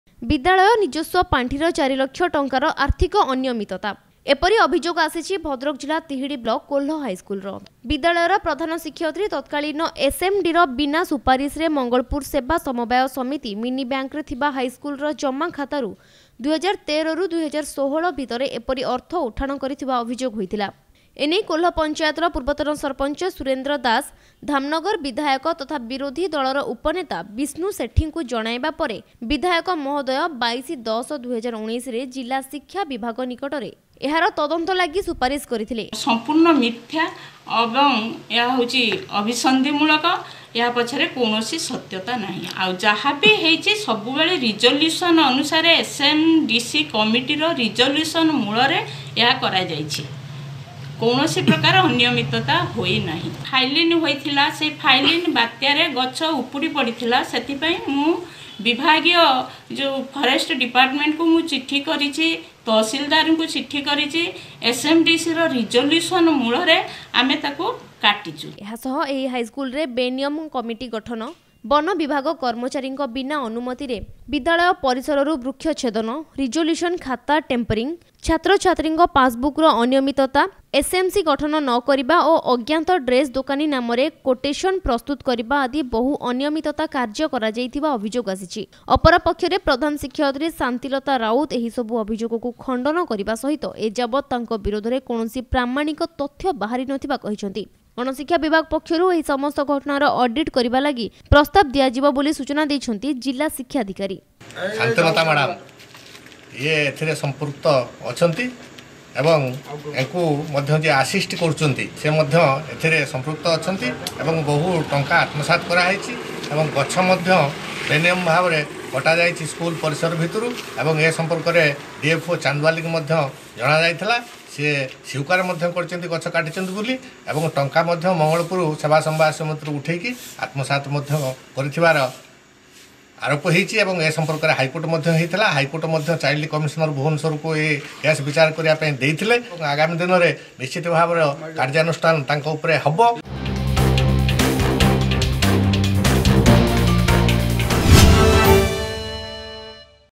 ବିଦ୍ୟାଳୟର ୪ ଲକ୍ଷ ଟଙ୍କାର ଆର୍ଥିକ ଅନିୟମିତତା ଅଭିଯୋଗ
ବିଦ୍ୟାଳୟ ନିଜସ୍ୱ ପାଣ୍ଠିର ୪ ଲକ୍ଷ ଟଙ୍କାର ଆର୍ଥିକ ଅନିୟମିତତା  । ଏପରି ଅଭିଯୋଗ ଆସିଛି ଭଦ୍ରକ ଜିଲ୍ଲା ତିହିଡି ବ୍ଲକ କୋହ୍ଲ ହାଇସ୍କୁଲର । ବିଦ୍ୟାଳୟର ପ୍ରଧାନ ଶିକ୍ଷୟତ୍ରୀ ତକ୍ରାଳୀନ ଏସଏମଡିସିର ବିନା ସୁପାରିଶରେ ମଙ୍ଗଳପୁର ସେବା ସମବାୟ ସମିତି ମିନିବ୍ୟାଙ୍କରେ ଥିବା ହାଇସ୍କୁଲର ଜମାଖାତାରୁ ୨୦୧୩ ରୁ ୨୦୧୬ ଭିତରେ ଏପରି ଅର୍ଥ ଉଠାଣ କରିଥିବା ଅଭିଯୋଗ ହୋଇଥିଲା । ଏନେଇ କୋହ୍ଲ ପଚାଂୟତର ପୂର୍ବତନ ସରପଂଚ ସୁରେନ୍ଦ୍ର ଦାସ ଦାମନଗର ବିଧାୟକ ତଥା ବିରୋଧି ଦଳର ଉପନେତା ବିଷ୍ଣୁ ସେଠାଙ୍କୁ ଜଣାଇବା ପରେ ବିଧାୟକ ମହୋଦୟ ୨୨.୧୦.୨୦୧୯ ରେ ଜିଲ୍ଲା ଶିକ୍ଷା ବିଭାଗ ନିକଟରେ ଏହାର ତଦନ୍ତ ଲାଗି ସୁପାରିଶ କରିଥିଲେ । ଏହାସହ ଏହି ହାଇସ୍କୁଲରେ ବେନିୟମ କମିଟି ଗଠନ, ବନବିଭାଗ କର୍ମ•ରୀଙ୍କ ବିନା ଅନୁମତିରେ ପରିସରରୁ ବୃକ୍ଷଛେଦନ, ରିଜୋଲୁ୍ୟସନ ଖାତା ଟେମ୍ପରି, ଛାତ୍ରଛାତ୍ରୀଙ୍କ ପାସବୁକ ର ଅନିୟମିତତା, ଏସଏମସି ଗଠନ ନକରିବା ଓ ଅଜ୍ଞାତ ଡ୍ରେସ ଦୋକାନୀ ନାମରେ କୋଟେସନ ପ୍ରସ୍ତୁତ କରିବା ଆଦି ବହୁ ଅନିୟମିତା କାର୍ଯ୍ୟ କରାଯାଇଥିବା ଅଭିଯୋଗ ଆସିଛି । ଅପରପକ୍ଷରେ ପ୍ରଧାନ ଶିକ୍ଷୟତ୍ରୀ ଶାନ୍ତିଲତା ରାଉତ ଏହିସବୁ ଅଭିଯୋଗକୁ ଖଣ୍ଡନ କରିବା ସହିତ ଏଯାବତ ତାଙ୍କ ବିରୋଧରେ କୌଣସି ପ୍ରମାଣିକ ତଥ୍ୟ ବାହାରି ନଥିବା କହିଛନ୍ତି ।  ଗଣଶିକ୍ଷା ବିଭାଗ ପକ୍ଷରୁ ଏହି ସମସ୍ତ ଘଟଣାର ଅଡିଟ୍ କରିବା ଲାଗି ପ୍ରସ୍ତାବ ଦିଆଯିବ ବୋଲି ସୂଚନା ଦେଇଛନ୍ତି ଜିଲ୍ଲା ଶିକ୍ଷାଧିକାରୀ ।